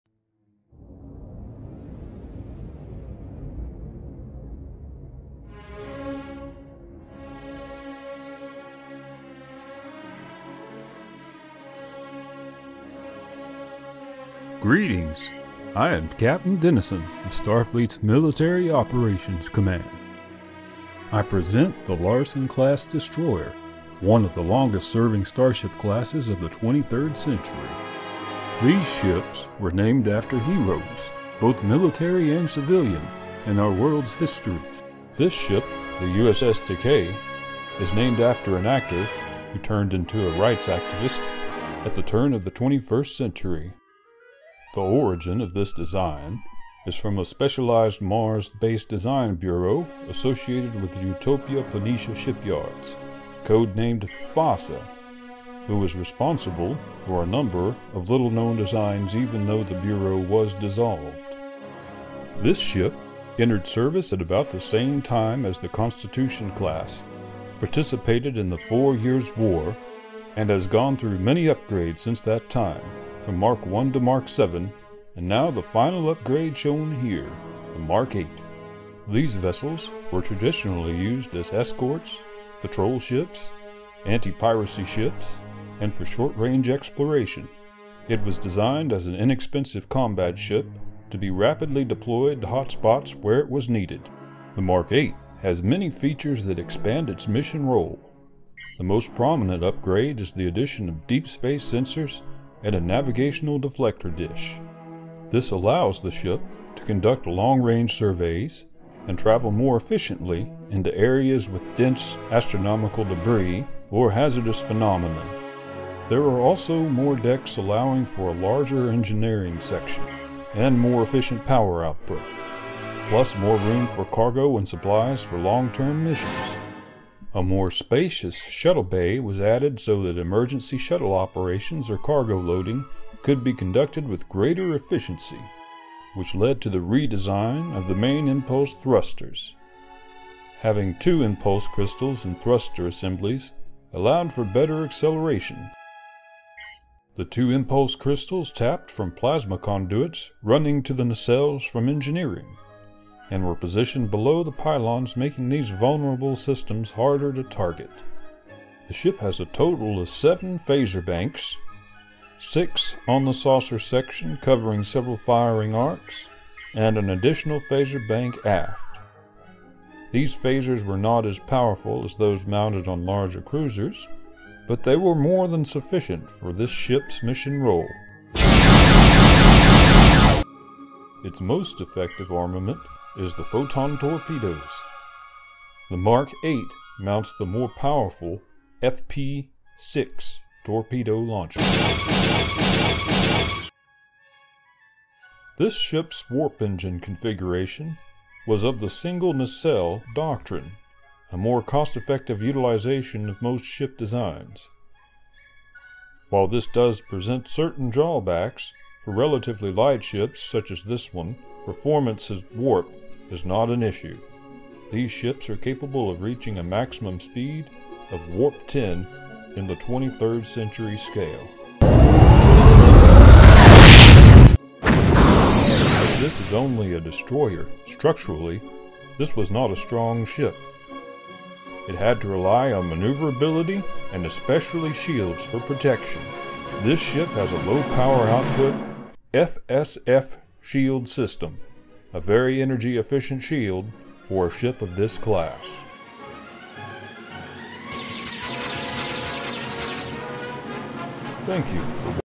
Starfleet's Workhorse Destroyer in the 23rd Century - Animated & Resurrected!
A lost star ship design from the 1980's table-top Star Trek Star Ship Tactical Combat Simulator game from FASA.  This ship has a strong cult following among fans. The Larson class originally was an old series era ship that has been upgraded several times. This is perhaps the final iteration; the Mark VIII.

Will publish a static page with graphics when I have created a lost starships website of my own. This is a fun little destroyer!